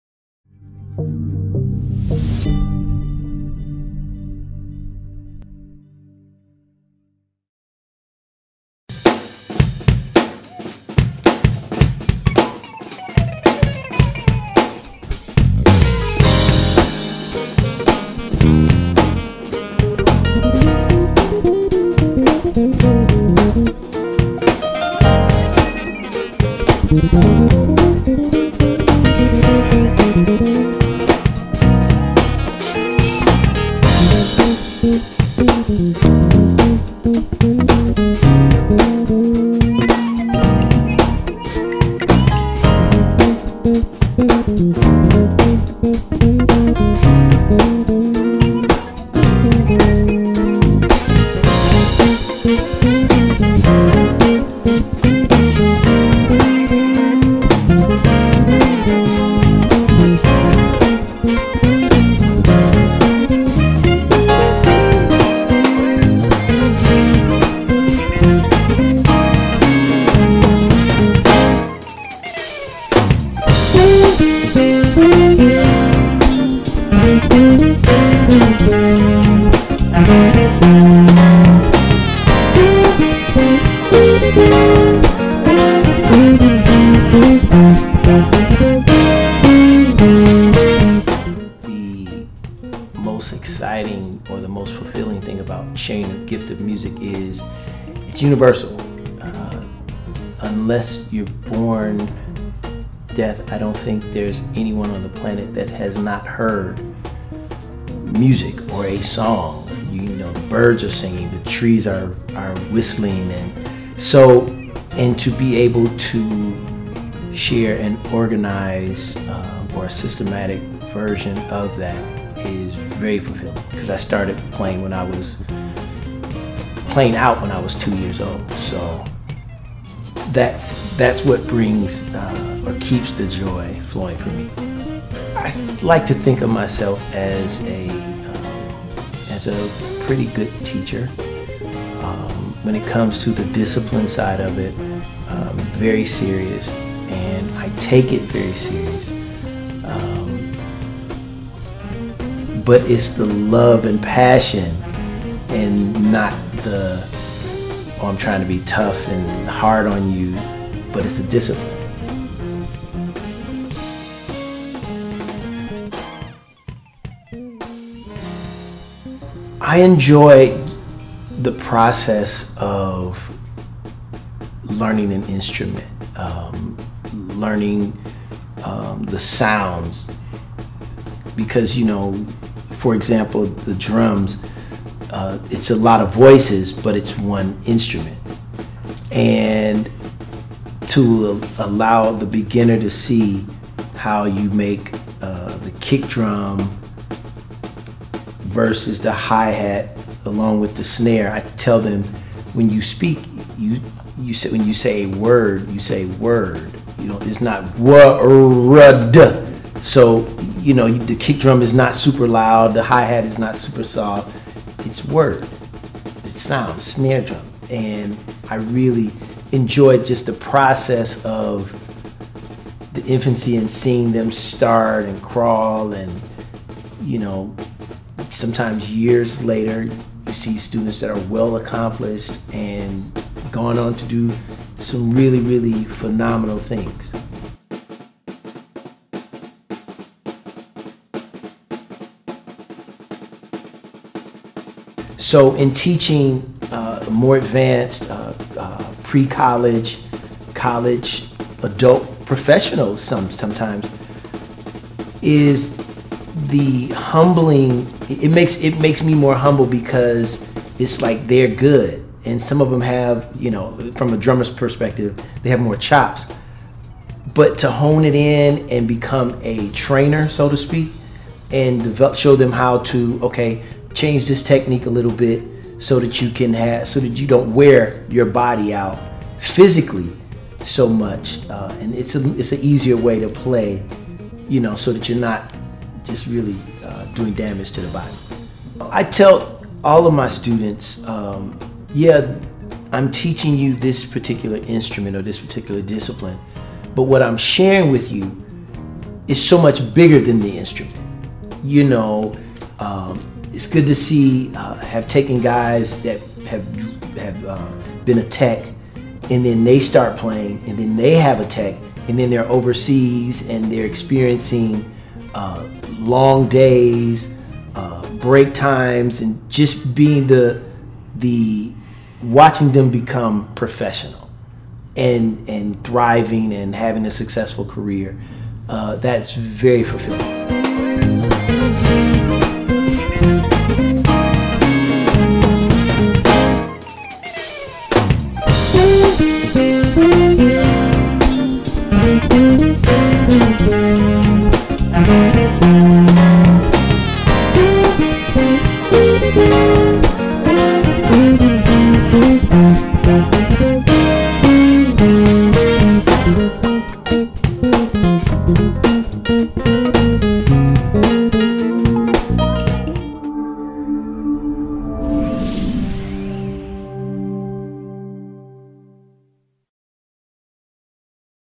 Meet MAI percussion instructor Felix Pollard
Meet our percussion instructor felixpollard!!! MAI instructors have the heart of the community. Sharing music and changing lives is behind the mission and vision of MAI. We celebrate those who share on the behalf of MAI. @musicalartsinstitute